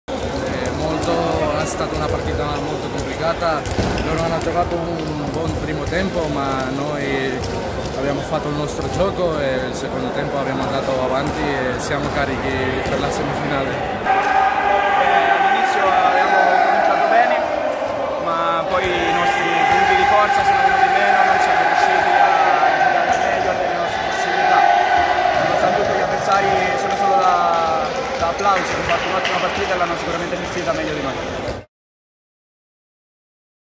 Finals 2022 | Serie A2 | Lanzara - Cingoli 38-32
Interviste post-gara a Enric Gonzalez Yanez (Lanzara) e Davide Ciattaglia (Cingoli).